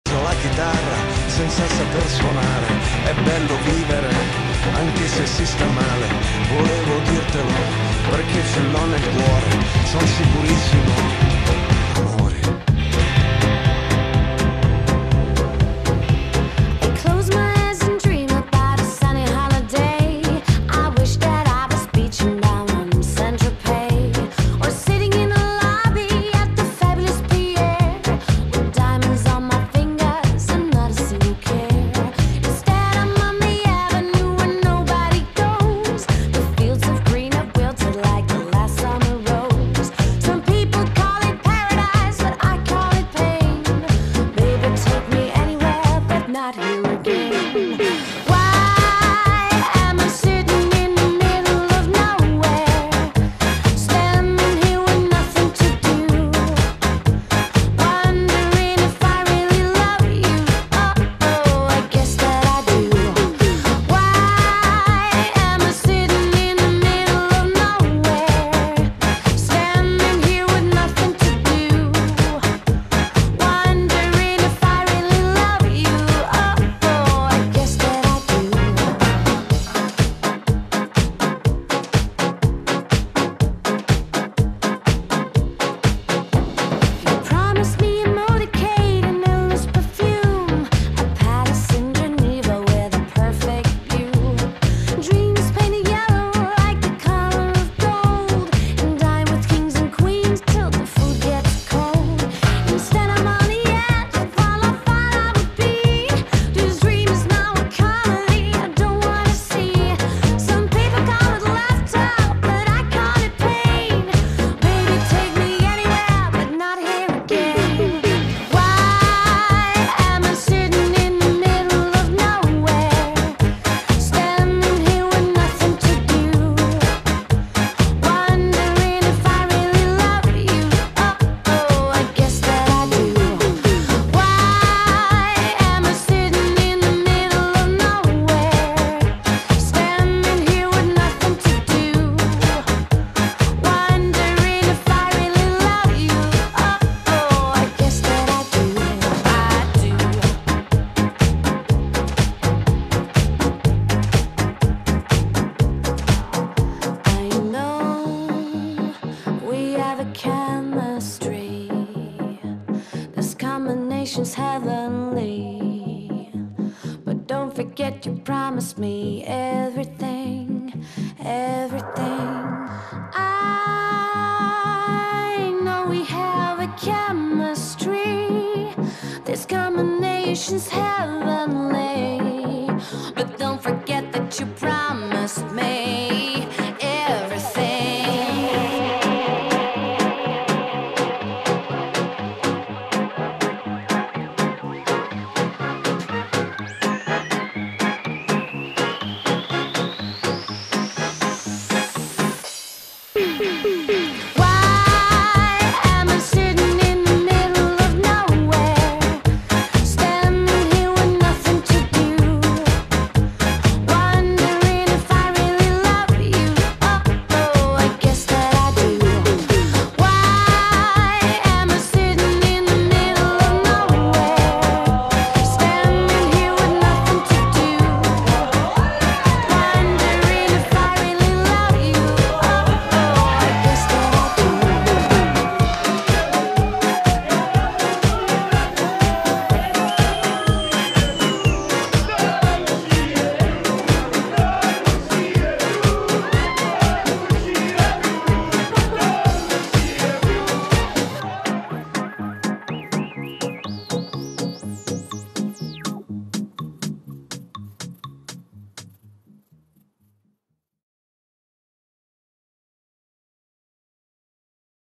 scenette da matrimonio
videoraccolta di scenette divertenti riprese durante il mio matrimonio :-)